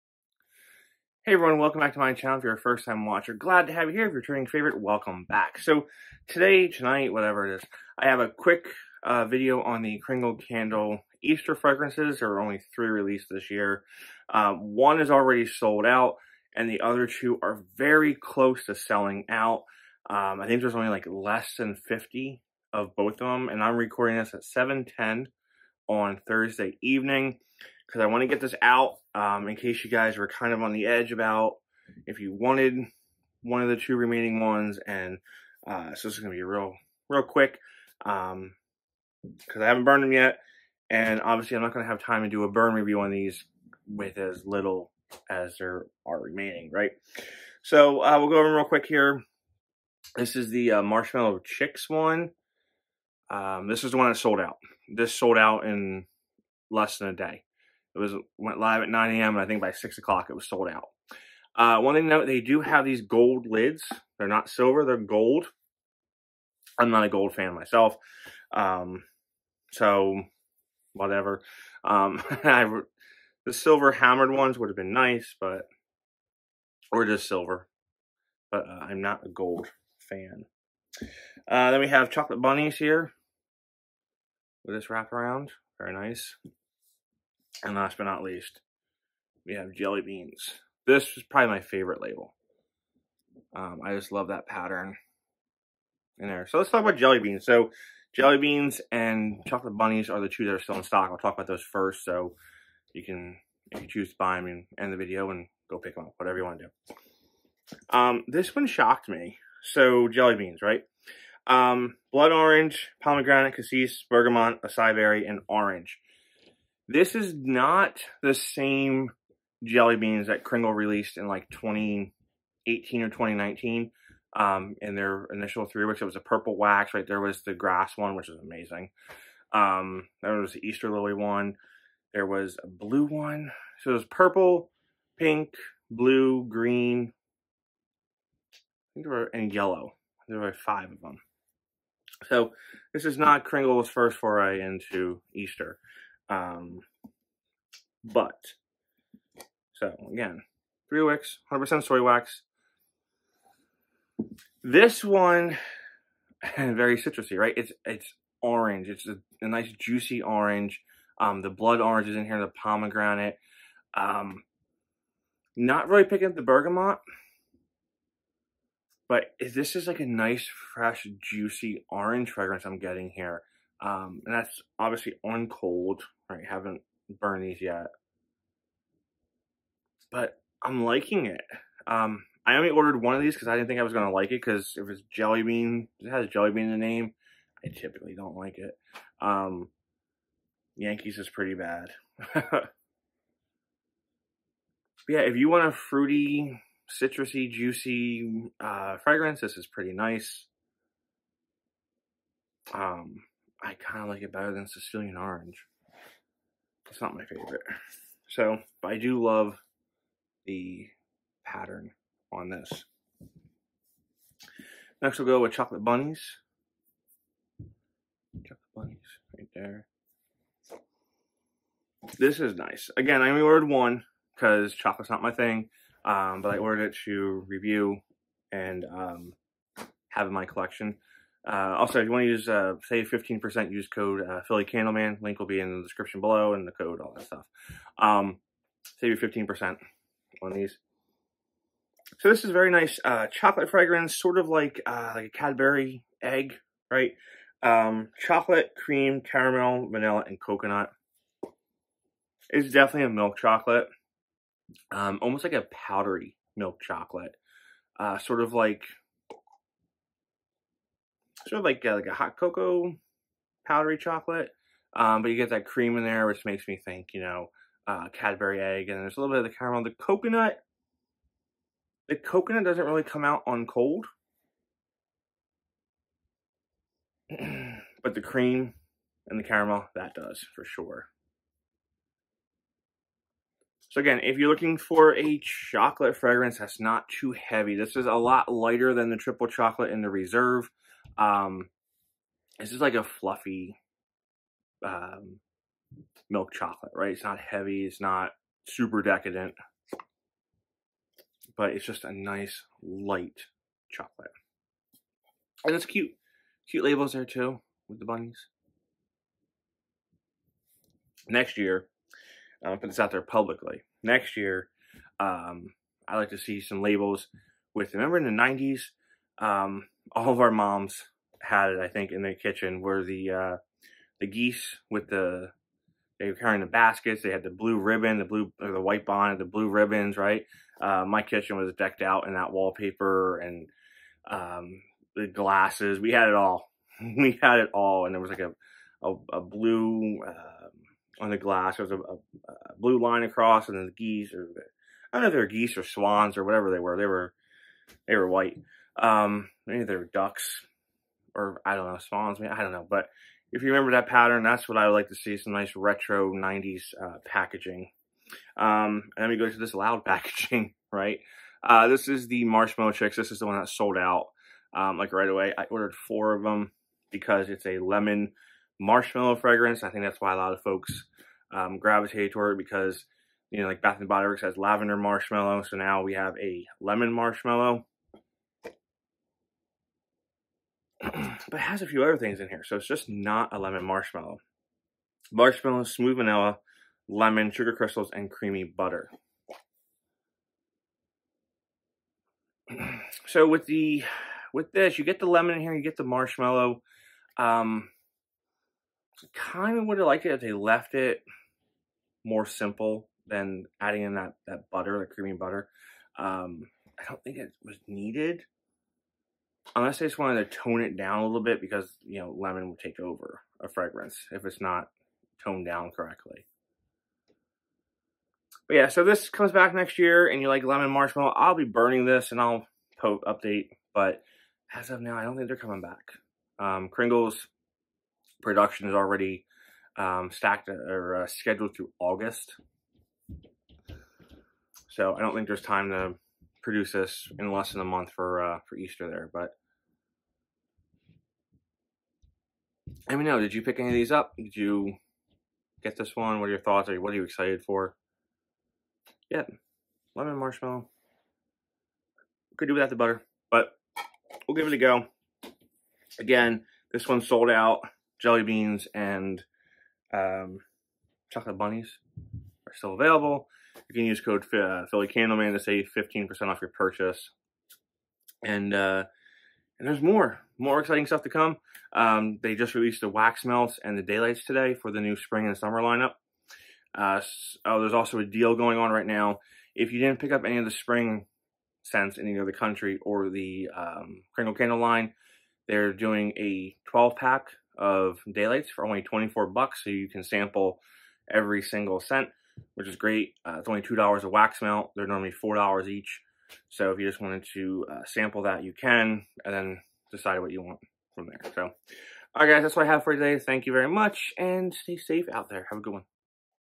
Kringle Candle Easter Collection Haul | 3 Wick | Spring 2023
Kringle Candle Easter Collection Haul

Save 15% by using code phillycandleman or shopping

Chocolate Bunnies – Walk into a candy shop with the smell of rich chocolate, cream, velvety caramel with vanilla & coconut...you've captured the essence of our Chocolate Bunnies!

Jelly Beans – Featuring blood orange, pomegranate and bergamot with a base of acai berry and orange, our Jelly Beans scented candles are deliciously fruity; you'll want to grab a handful!

Marshmallow Chicks – Marshmallow Chicks means springtime! Enjoy this sweet, sugary delight with flavorful aromas of lemon, fluffy marshmallow and comforting creamy butter!

Below are the fragrances for both Kringle and Country Candle lines. New this season is Country Candle fragrances are being poured in 100% soy wax!

Avocado Palm
T: Sea Salt, Fresh Air, Green Melon
M: Sun Ripened Avocado, Waterlily, Apple Blossom
B:Palm Leaf, Aloe Water, Beach Wood

Botanicals
T: Morning Dew, Rosewater, Orange Blossom
M: Lily of the Valley, Pink Jasmine, Hyacinth
B: Green Bamboo, Lilac Leaves, Meadow Grass

Mon Amour
T: Caramel Strands, Sugar Crystals, Vanilla Orchid
M: Star Jasmine, Oakmoss, Crimson Blooms
B: Red Amber, Gilded Musk, Sandalwood

Salt Water Taffy
T: Sugar, Vanilla
M: Strawberry, Sea Salt
B: Fruit, Sweet

Sea Breeze
T: Ozone, Coconut
M: Fruit, Green
B: Coconut, Amber

Tea Time
T: Clove Bud, Zesty Orange, Cardamom
M: Star Anise, Steeped Tea Leaves, Cinnamon
B: Sandal Woods, Golden Honey

Blue Raspberry
T: Lime, Citron, Glazed Raspberries, Sweet Strawberries
M: Sugared Blueberries, Spun Cotton Candy
B: Dazzling Sugar Crystal Woods, Candied Musk

Coconut Blueberry Tart
T: Blueberry, Plum, Butter, Orange, Peach
M: Coconut, Cinnamon, Jasmine, Raspberry
B: Almond, Vanilla, Caramel, Musk

T: Chocolate Chip, Creamy Fudge, Whipped Cream
M: Milk, Vanilla Orchid, Praline
B: Tonka Bean, Vanilla, Musk

Cosmic Cupcakes
T: Candied Lemon, Orange Juice, Lime Zest, Red Berries
M: Coconut Milk, Cocoa Powder, Buttery Caramel
B: Sugar Crystals, Vanilla Buttercream Frosting

Grapefruit Rosemary
T: Sparkling Bergamot, Lemon Leaf, Juicy Pink Grapefruit
M: Wild Rosemary, White Lavender, Dewy Garden Greens
B: Creamy White Woods, Musk

Sweet Peach
T: Honey Drizzle, Whipped Cream, Apricot Nectar
M: Georgia Peach, Brown Sugar, Nutmeg
B: Tonka Bean, Madagascar Vanilla, Toasted Oats